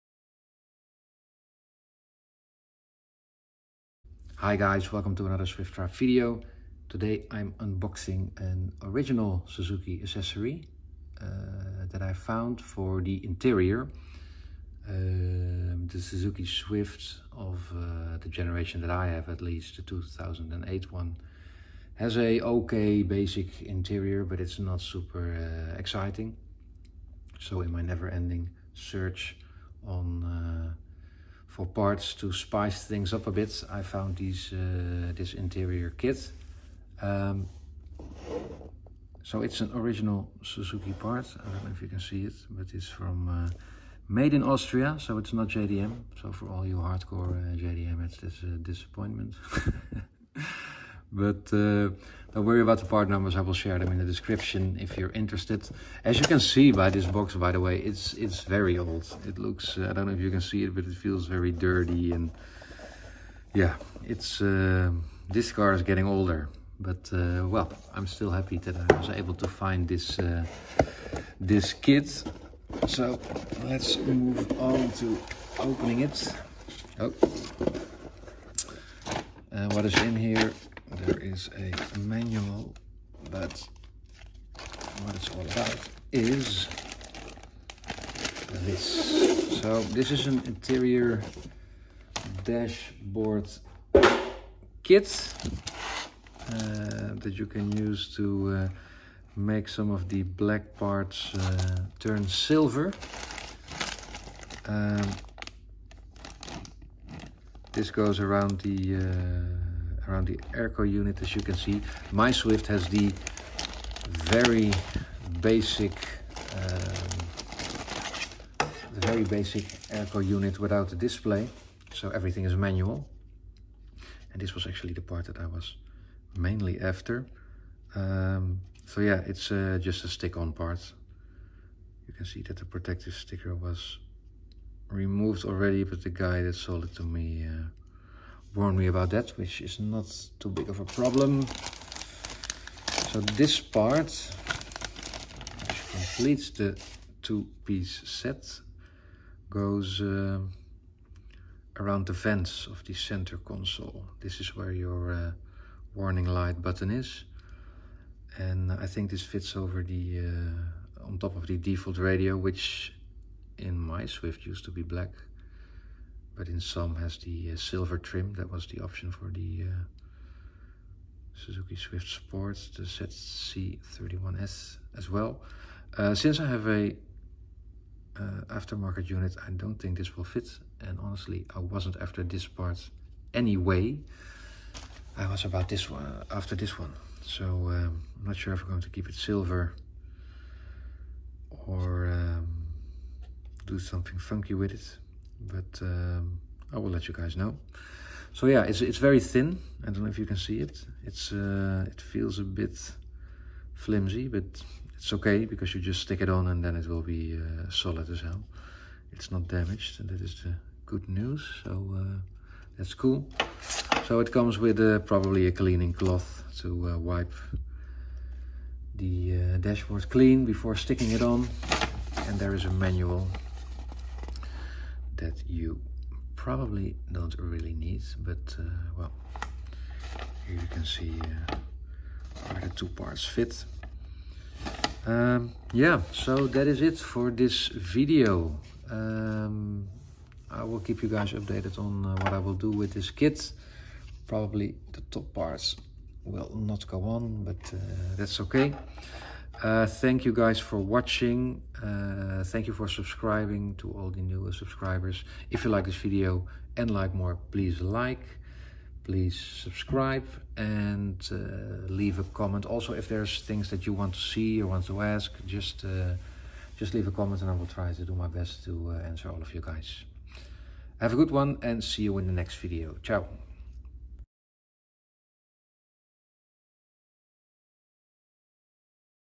Suzuki Swift center console trim unboxing
In this video you can see an unboxing of an OEM Suzuki accessory; a two-piece aluminium (look) center console trim set. Part number: 990E0-62J52-000